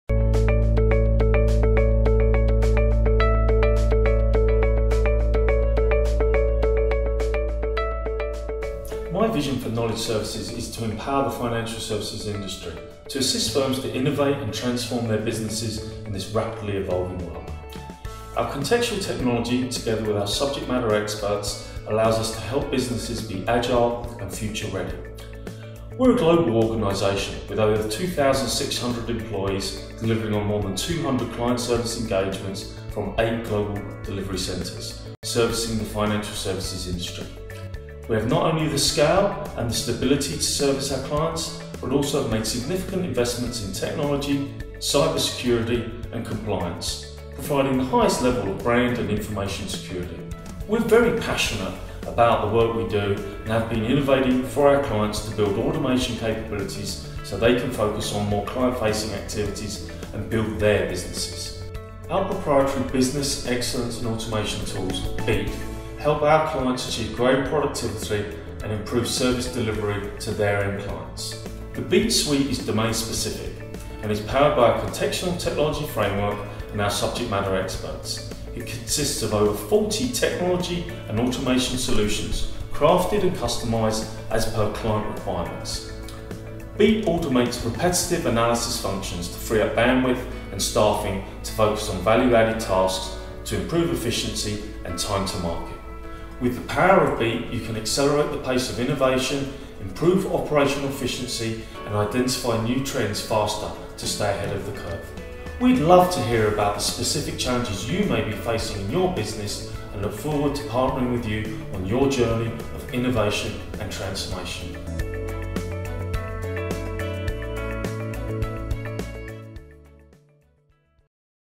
Robert King on his Vision for Acuity Knowledge Partners
Robert King, Executive Director and Global Head on his vision for Acuity Knowledge Partners